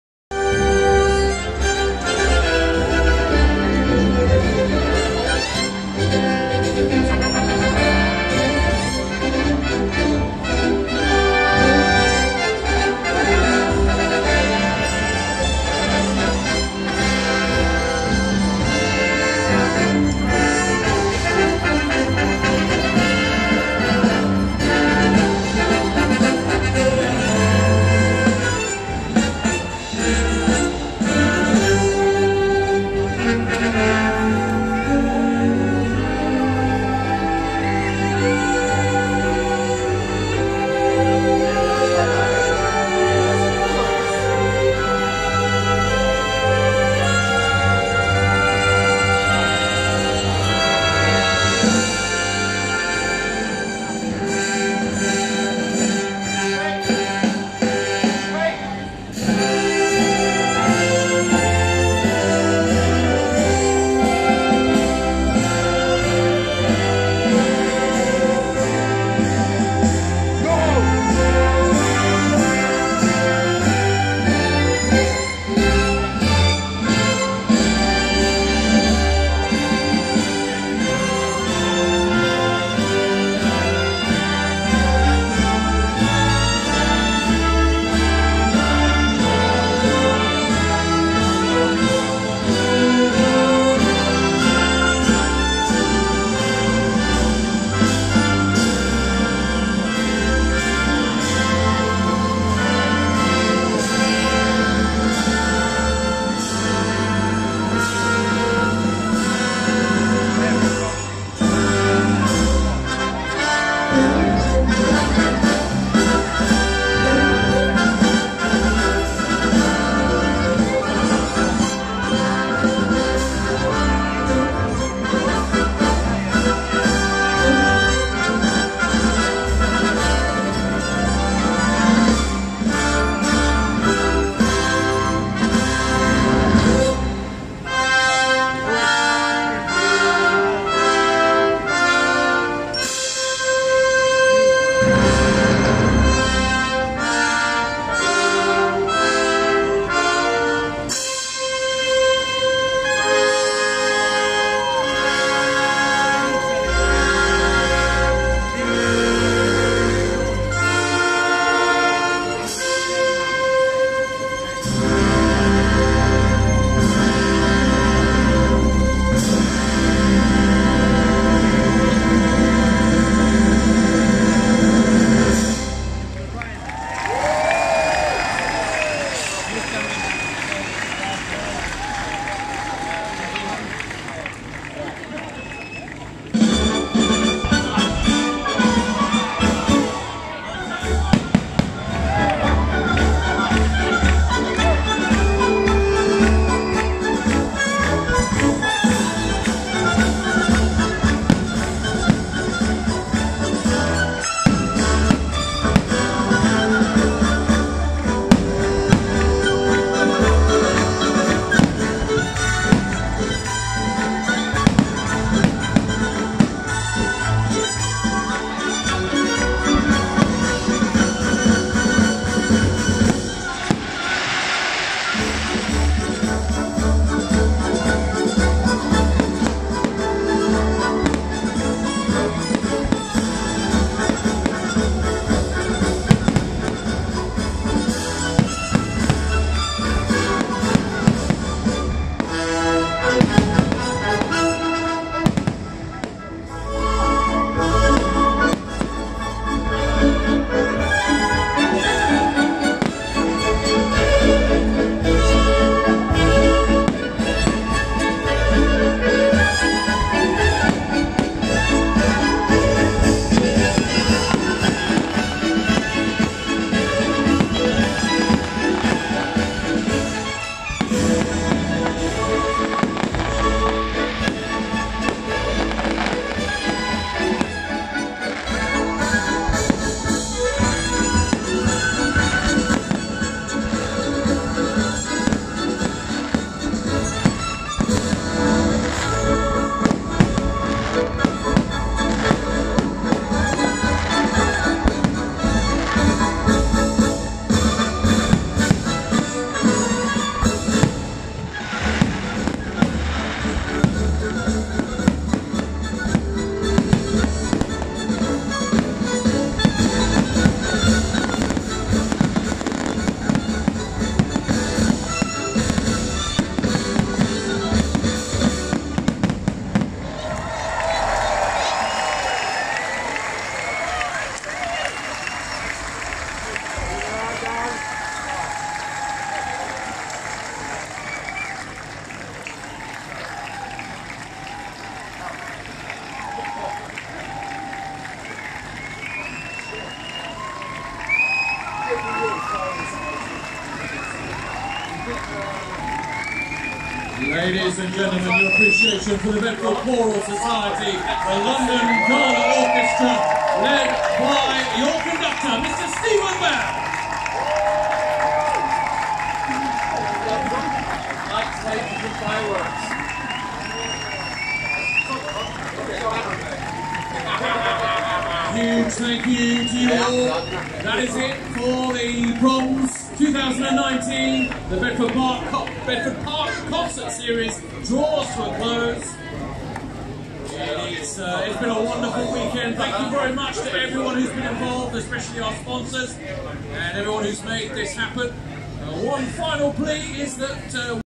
The Bedford Proms in The Park dramatic Finale on Sunday 28th July 2019
The ending of a great night of The Bedford Proms in The Park Finale 2019 with amazing lasers and fireworks   Please don't forget to like and subscribe and turn on notifications so you will know when we release new videos